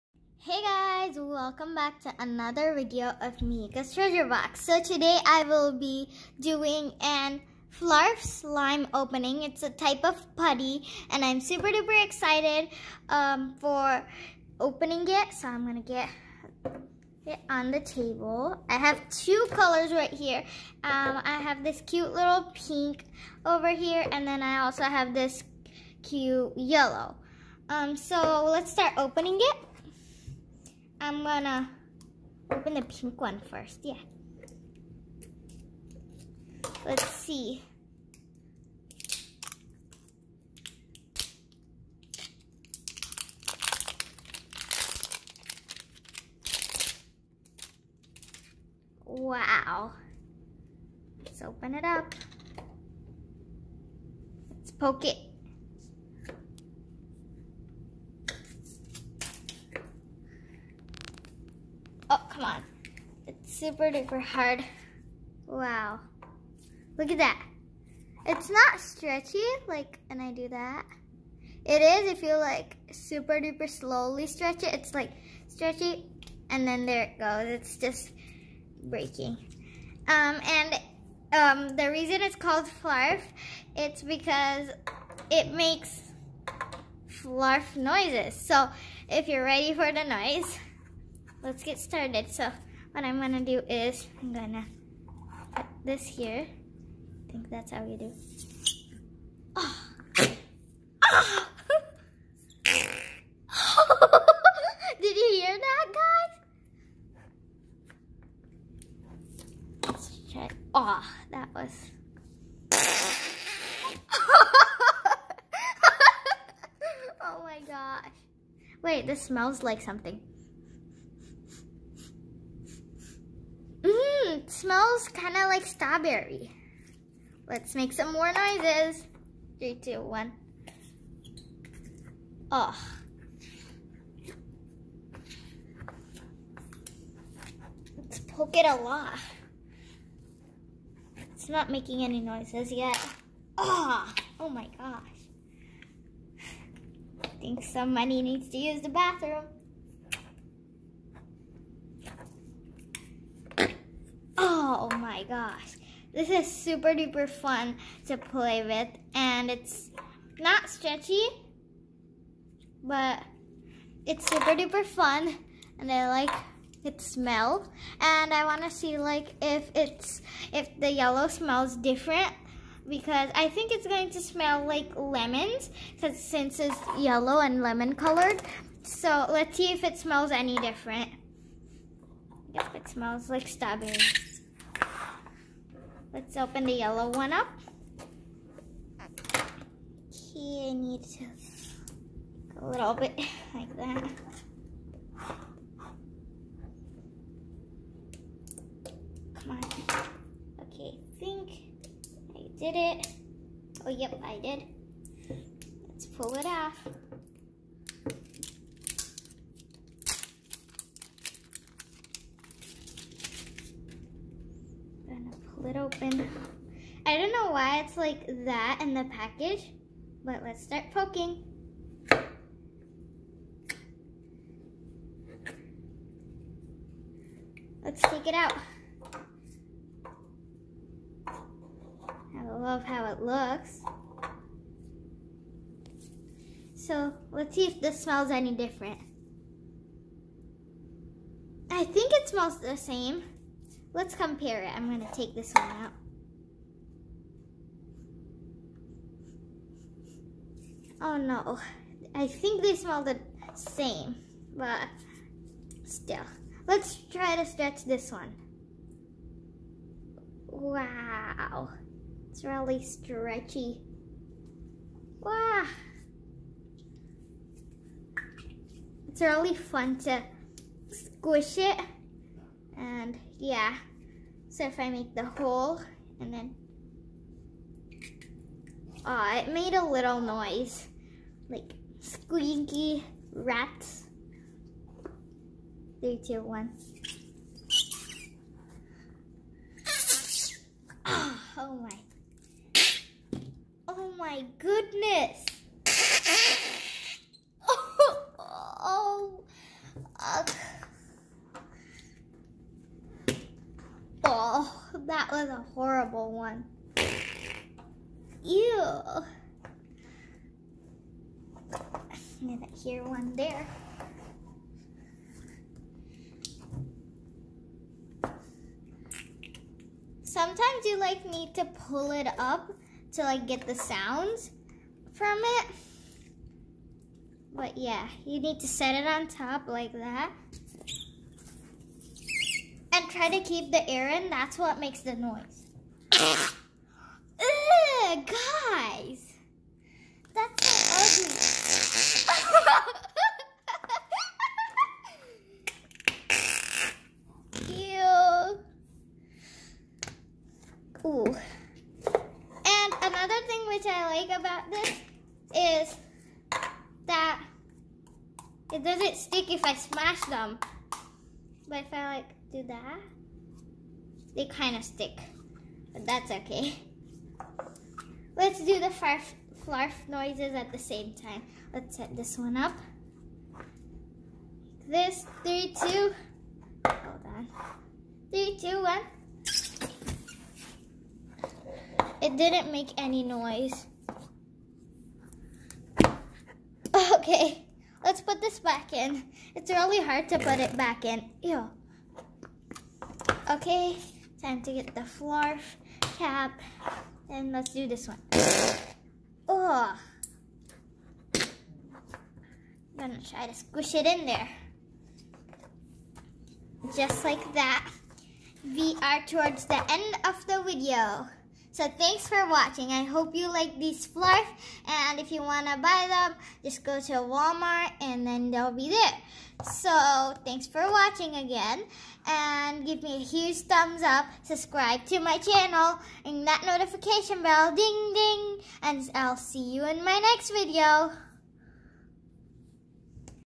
Flarp noise putty opening
Hey guys I had a lot of fun unboxing the Yellow and pink Flarp noise putty and here is  the link to my part 2 Flarp noise putty opening: